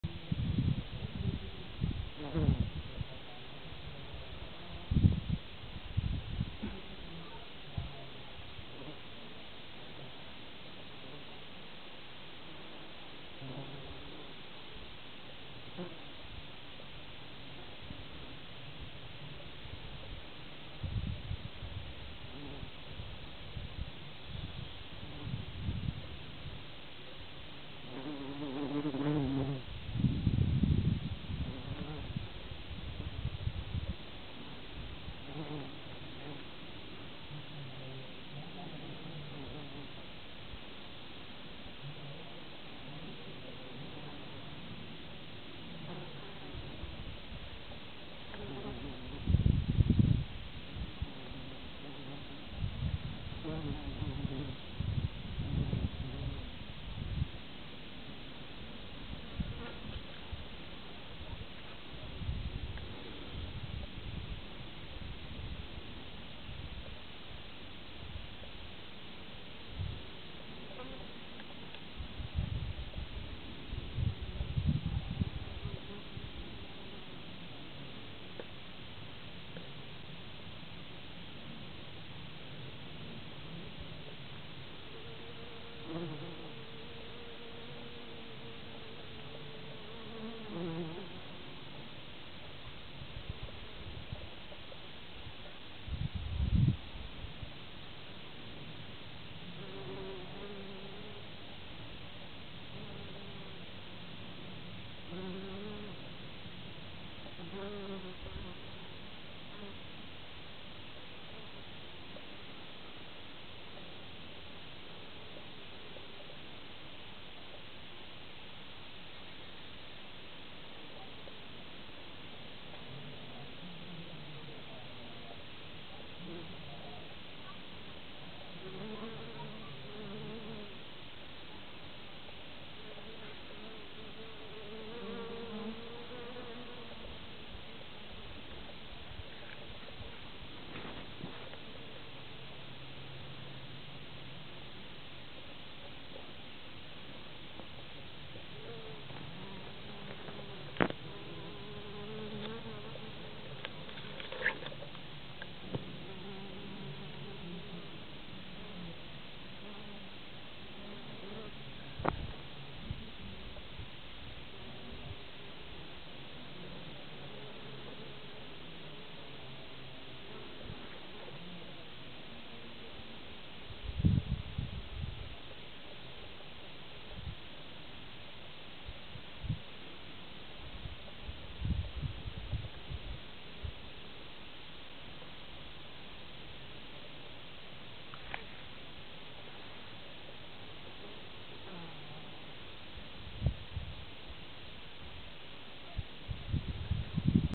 ..."hardworking" insects in my herb garden...Austria 2010
..."hard-working" insects in my garden...Austria 2010 mei bio-garchtl...:)! Permakultur in Linz